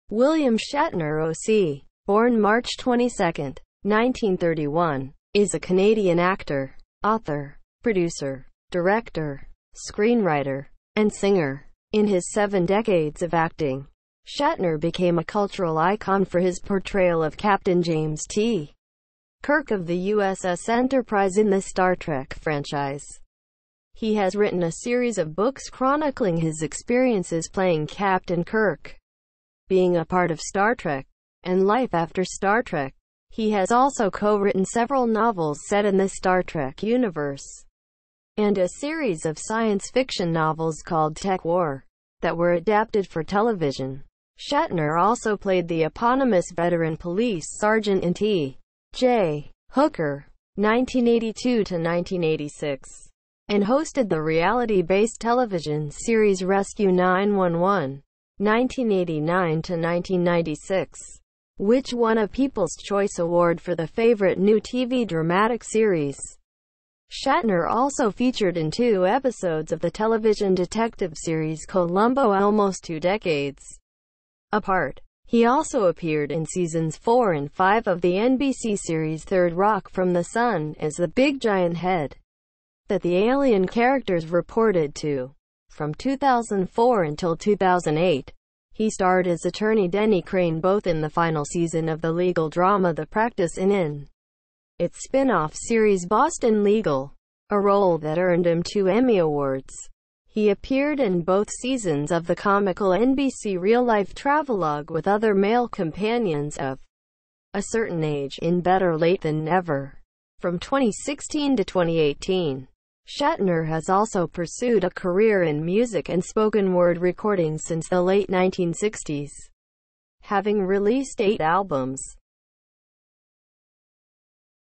William Shatner Biography
This video is about William Shatner Biography in English.
William Shatner OC (born March 22, 1931) is a Canadian actor, author, producer, director, screenwriter, and singer. In his seven decades of acting, Shatner became a cultural icon for his portrayal of Captain James T. Kirk of the USS Enterprise in the Star Trek franchise. He has written a series of books chronicling his experiences playing Captain Kirk, being a part of Star Trek, and life after Star Trek. He has also co-written several novels set in the Star Trek universe, and a series of science fiction novels called TekWar, that were adapted for television.
Shatner also played the eponymous veteran police sergeant in T. J. Hooker (1982–1986) and hosted the reality-based television series Rescue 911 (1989–1996), which won a People's Choice Award for the Favorite New TV Dramatic Series. Shatner also featured in two episodes of the television detective series Columbo almost two decades apart. He also appeared in seasons 4 and 5 of the NBC series 3rd Rock from the Sun as the "Big Giant Head" that the alien characters reported to. From 2004 until 2008, he starred as attorney Denny Crane both in the final season of the legal drama The Practice and in its spinoff series Boston Legal, a role that earned him two Emmy Awards. He appeared in both seasons of the comical NBC real-life travelogue with other male companions "of a certain age" in Better Late Than Never, from 2016 to 2018. Shatner has also pursued a career in music and spoken-word recordings since the late 1960s, having released eight albums.